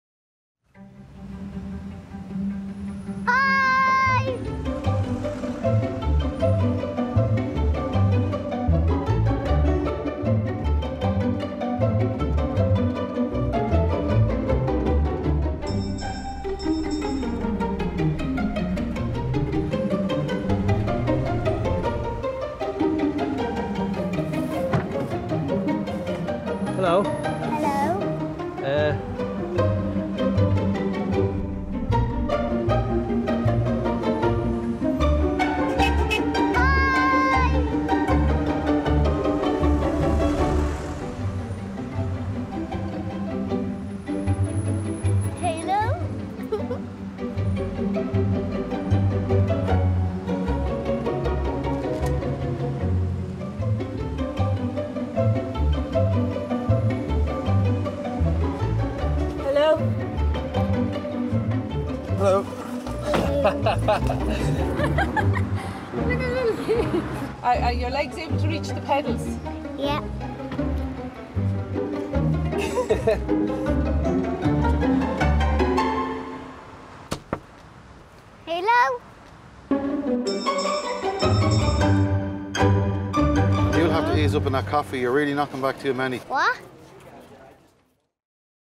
Say Hailo to the 3 year old taxi driver…
We've just turned 3! To celebrate we thought we’d have a little fun with our customers by introducing them to the youngest driver in the world, our very own 3 year old taxi driver.

Created by the Guns or knives team:)